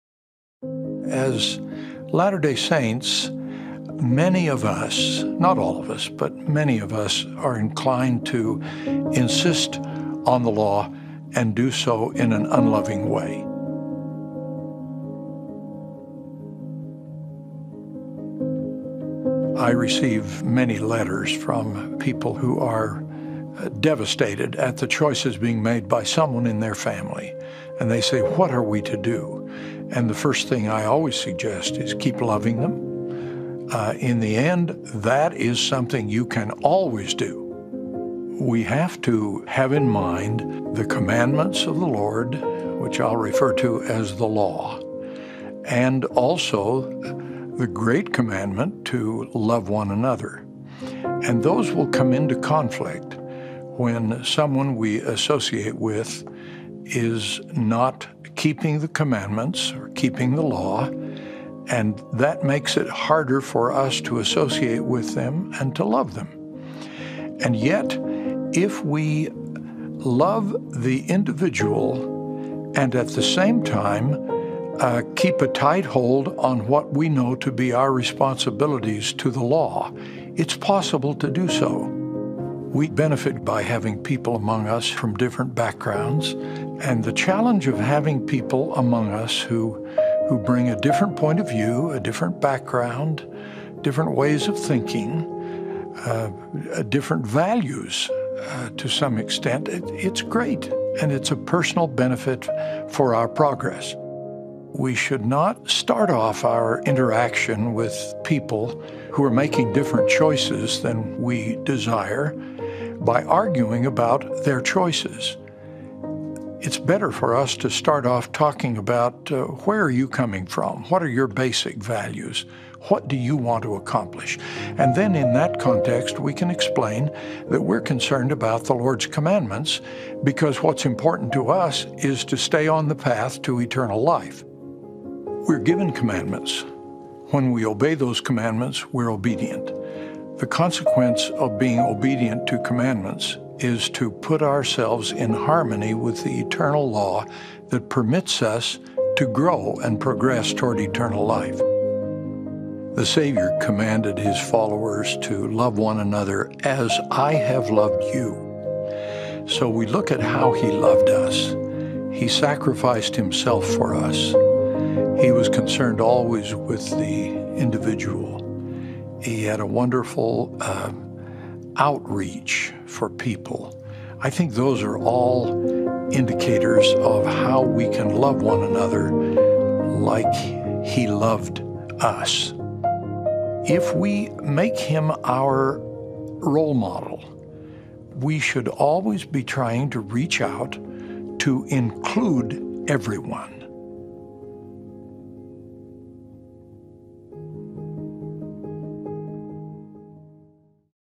OT 05.2 Love and Law
“Love and Law” from the LDS Media Library. In the video Elder Dallin H. Oaks counsels how he should love those who are not following the laws of God.